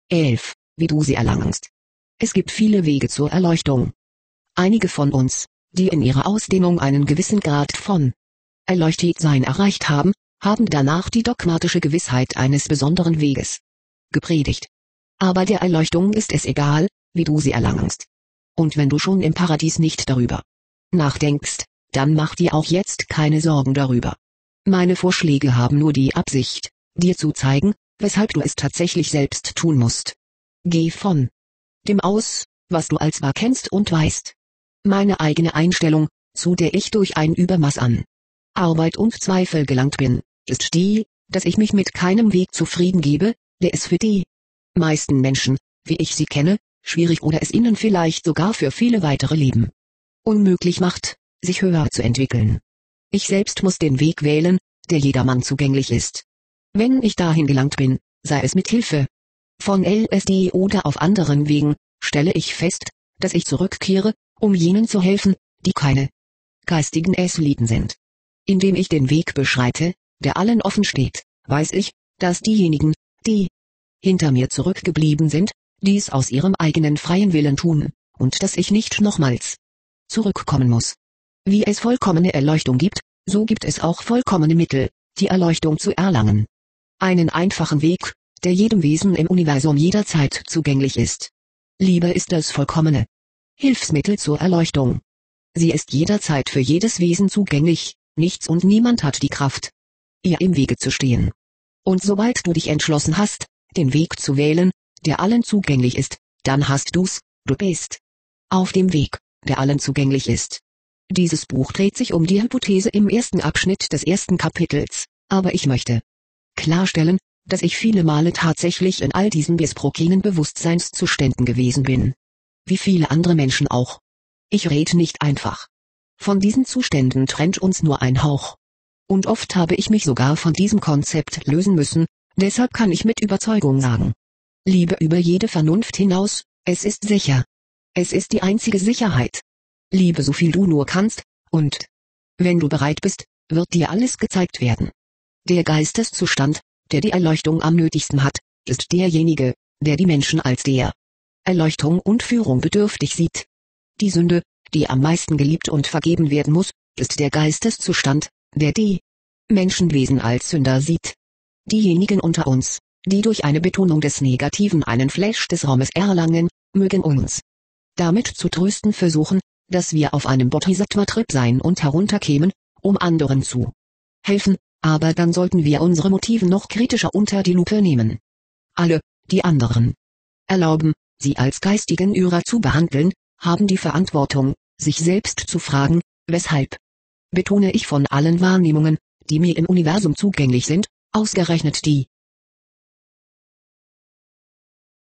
027 Thaddeus Golas Der Erleuchtung ist es egal, wie du sie erlangst
Thaddeus Golas Der Erleuchtung ist es egal, wie du sie erlangst
das pdf:

ausschnitte "das buch des lichts" : (SO WIRST DU UNSTERBLICH!)